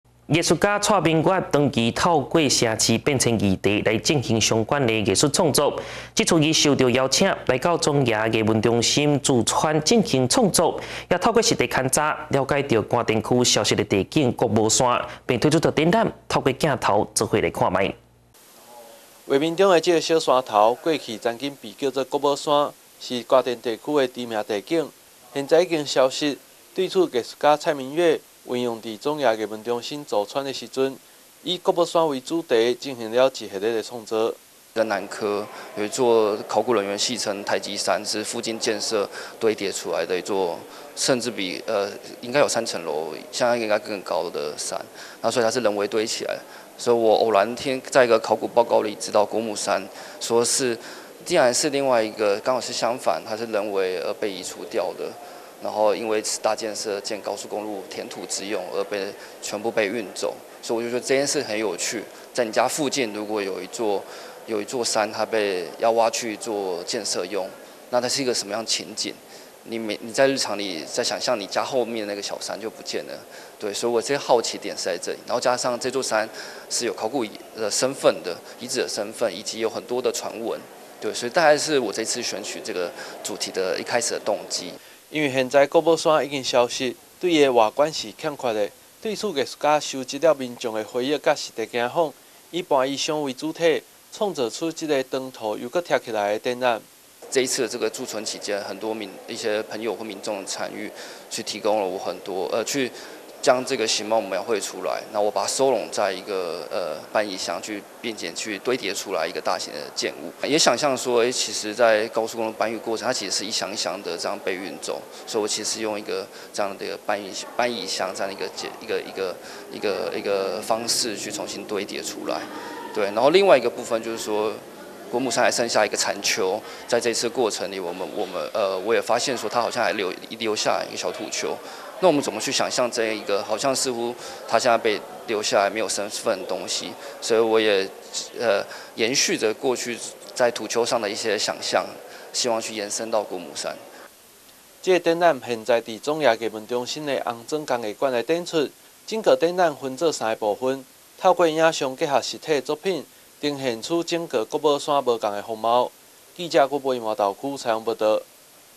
蔡明岳個展 透過藝術找尋官田消失國母山-新永安新聞
藝術家蔡明岳 長期透過城市變遷議題來進行相關的藝術創作 這次他受邀來到總爺藝文中心 駐村進行創作 他也透過實地勘察 了解官田區消失的地景 國母山 並推出展覽 透過鏡頭一起來看看